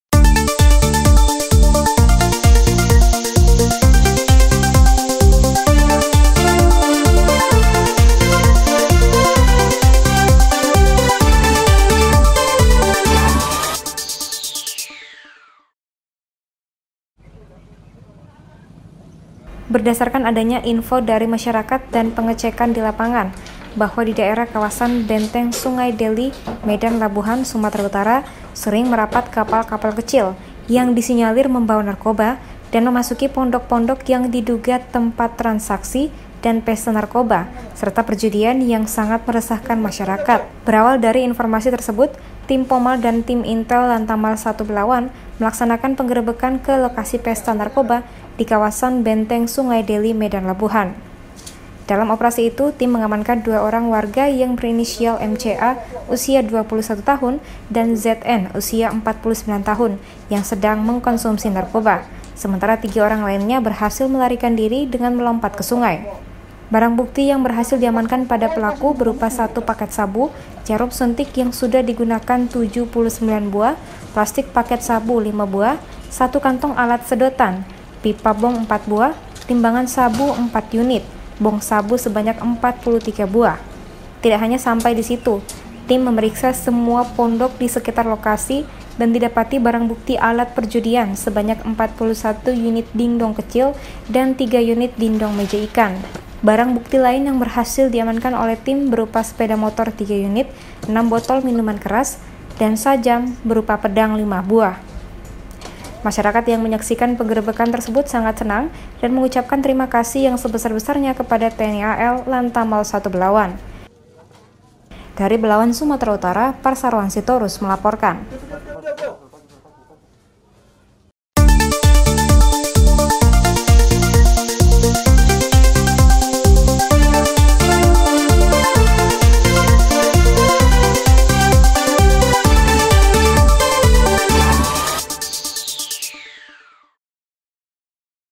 POMAL DAN TIM INTEL LANTAMAL 1 BELAWAN GREBEK LOKASI PESTA NARKOBA DAN PERJUDIAN
BERDASARKAN ADANYA INFO DARI MASYARAKAT DAN PENGECEKKAN DI LAPANGAN / BAHWA DI DAERAH KAWASAN BENTENG SUNGAI DELI MEDAN LABUHAN SERING MERAPAT KAPAL KAPAL KECIL YANG DI SINYALIR MEMBAWA NARKOBA DAN MEMASUKI PONDOK PONDOK YANG DI DUGA TEMPAT TRANSAKSI DAN PESTA NARKOBA SERTA PERJUDIAN YANG SANGAT MERESAHKAN MASYARAKAT / BERAWAL DARI INFORMASI TERSEBUT TIM POMAL DAN TIM INTEL LANTAMAL 1 BELAWAN MELAKSANAHKAN PENGGEREBEKKAN KE LOKASI PESTA NARKOBA KAWASAN BENTENG SUNGAI DELI / MEDAN LABUHAN //

DENGAN MELAKUKAN PENDADAKAN PADA LOKASI TERSEBUT TIM TELAH MENGAMANKAN 2 ORANG WARGA YANG BERINISIAL MCA USIA 21 TAHUN DAN ZN USIA 49 TAHUN YANG SEDANG MENGKONSUMSI NARKOBA / SEMENTARA 3 ORANG LAINNYA BERHASIL MELARIKAN DIRI DENGAN MELOMPAT KE SUNGAI //

BARANG BUKTI YANG BERHASIL DI AMANKAN PADA PELAKU BERUPA 1 PAKET SABU / JARUM SUNTIK YANG SUDAH DI GUNAKAN 79 BUAH / PLASTIC PAKET SABU 5 BUAH / 1 KANTONG ALAT SEDOTAN / PIPA BONG 4 BUAH / TIMBANGAN SABU 4 UNIT / BONG SABU SEBANYAK 43 BUAH / TIDAK HANYA SAMPAI DI SITU / TIM MEMERIKSA SEMUA PONDOK DI SEKITAR LOKASI DAN DI DAPATI BARANG BUKTI ALAT PERJUDIAN SEBANYAK 41 UNIT DINDONG KECIL / 3 UNIT DINDONG MEJA IKAN //

BARANG BUKTI LAIN YANG BERHASIL DI AMANKAN OLEH TIM BERUPA SEPEDA MOTOR 3 UNIT / 6 BOTOL MINUMAN KERAS / SAJAM BERUPA PEDANG 5 BUAH //

MASYARAKAT YANG MENYAKSIKAN PENGGEREBEKKAN TERSEBUT SANGAT SENANG DAN MENGUCAPKAN TERIMA KASIH YANG SEBESAR BESARNYA KEPADA TNI AL LANTAMAL 1 BELAWAN YANG TELAH MEMBANTU KAMI MENGHENTIKAN KEGIATAN TERLARANG YANG SANGAT MERESAHKAN MASYARAKAT SEKITAR SUNGAI DELI MEDAN LABUHAN / PUNGKAS DANPOMAL LANTAMAL 1 LETKOL LAUT ( PM ) DESY ARNAZ S.A.P. MTr OPSLA YANG BERTINDAK SEBAGAI KATIM DALAM AKSI TERSEBUT ///